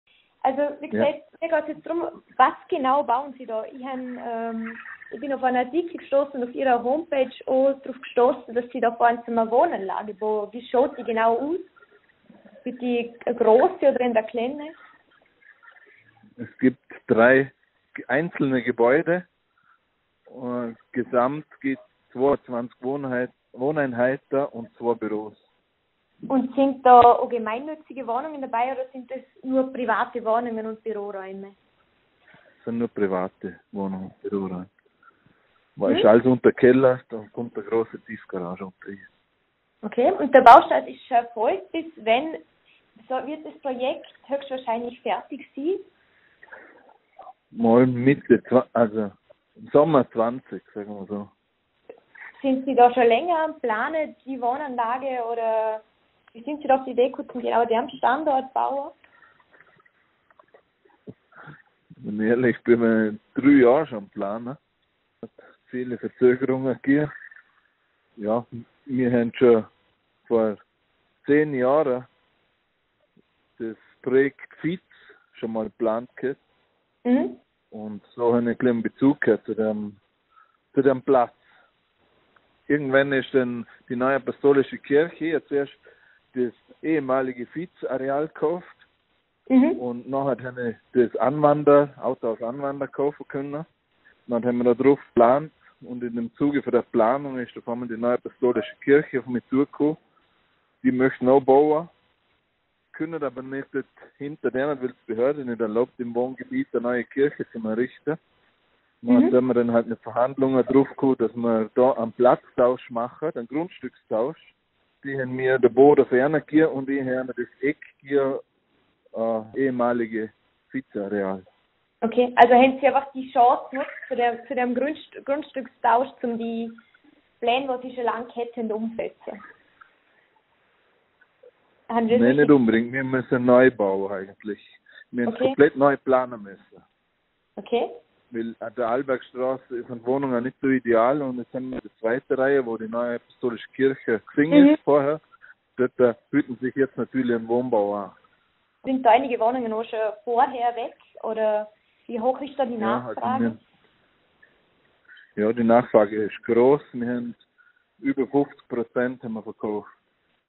Bregenz-Rieden: Baumeister Peter Winder zum Bau der Wohnanlage
Nach Abriss der neuapostolische Kirche Bregenz-Rieden soll auf dem Grundstück eine neue Wohnanlage entstehen. Baumeister Peter Winder über das Bauprojekt.